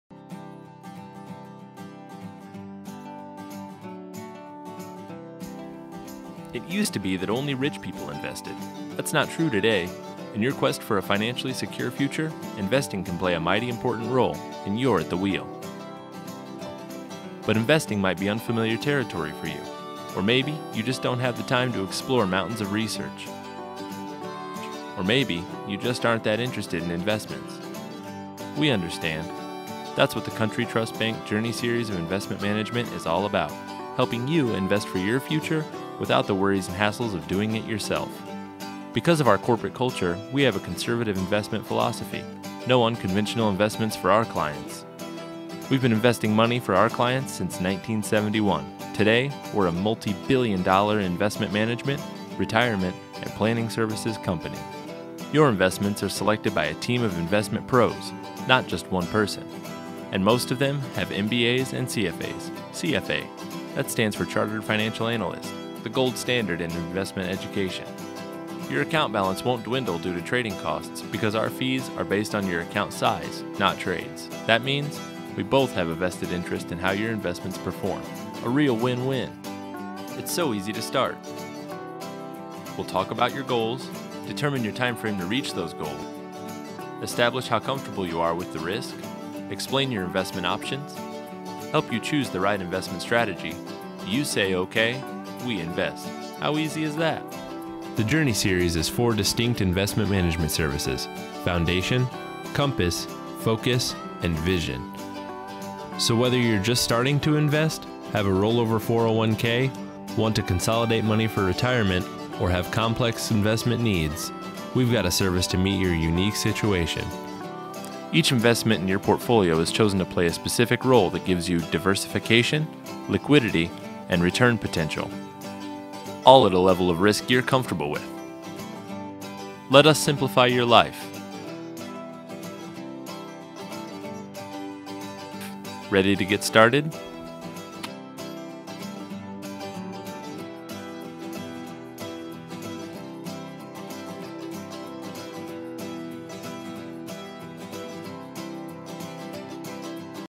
journeySeries update2015
Our Journey Series of Investment Management can simplify your life whether you are starting to invest, have a rollover 401(k), or have complex investment needs.